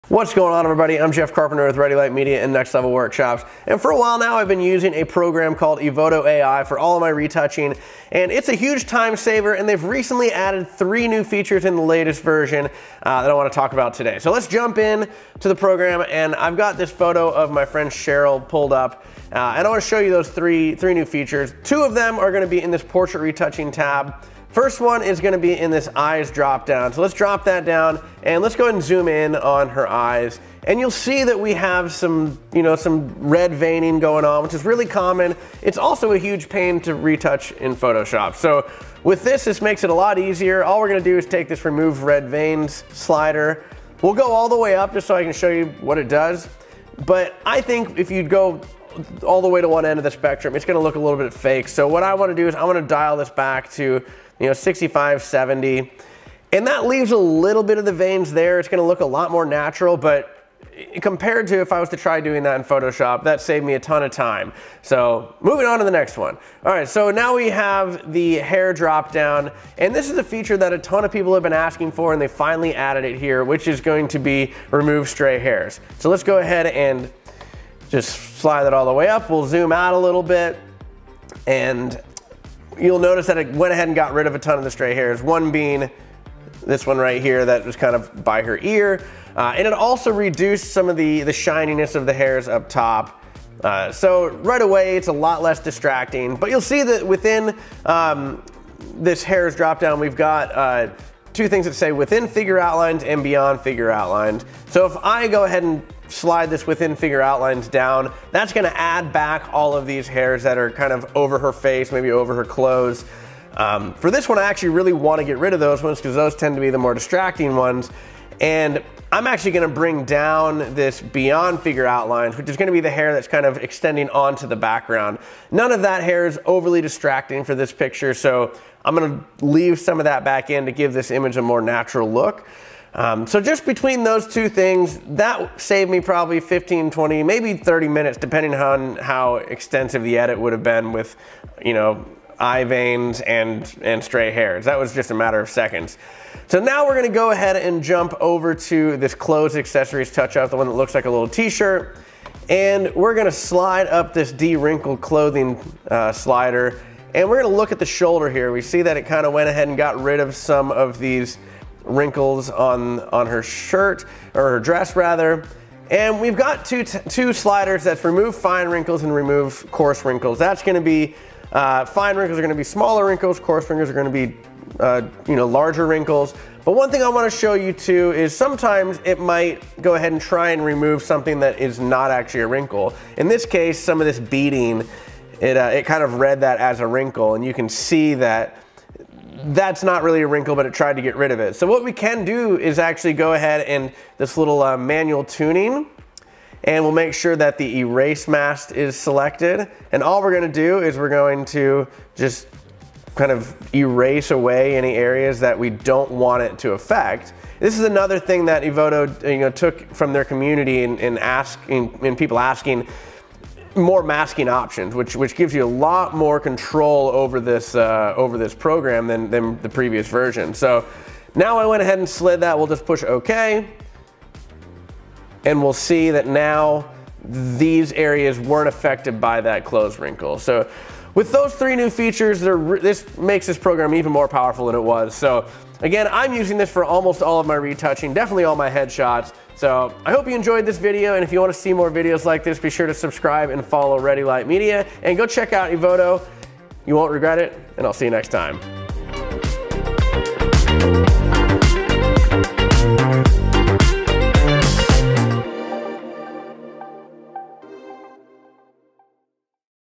Remove Stray Hairs in Seconds with EVOTO Ai!
EVOTO Ai recently added a few new features that I am extremely excited about! Save yourself time on editing, and allow more time for shooting with Ai photo editing.

Ok, so what are the features? Well let me tell you…

Red vein removal in eyes
Removing veins in eyes is always a really tedious task, but with this new feature, it literally takes less than a second!

Stray hair removal
This is a feature that a lot of people have been asking for, and it’s honestly really good! Not only does it save a ton of time tediously editing hairs away, it gives you extra control of hairs within the subject outline, and also beyond it.

Clothes de-wrinkle
This one I don’t need all that often because I try to tell my clients to have their clothes prepared before the shoot, but when I do need it, I hate editing wrinkles out. With this new feature,  it’s such a breeze.

What I am most excited about with this new update is that I am needing other editing programs less and less. Eventually we probably won’t even need photoshop to edit portraits and headshots.